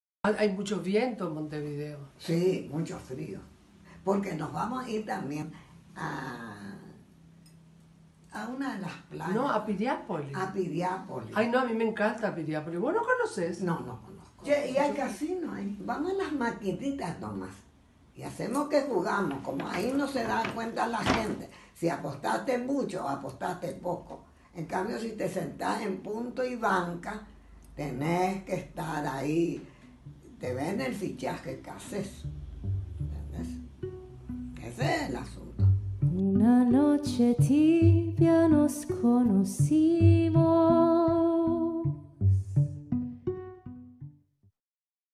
Las Herederas de Martinessi planean viaje a Piriápolis (Uruguay)
Asunción. Paraguay. 8 de agosto de 2018.

El sábado 11 de agosto de 2018, el Festival Piriápolis de Película (Uruguay) presenta el avant premiere de "Las herederas", con presencia de Ana Brun, Margarita Irún y María Martins (Pituca)

Estreno en el circuito comercial de cines de Uruguay: 16 de agosto de 2018.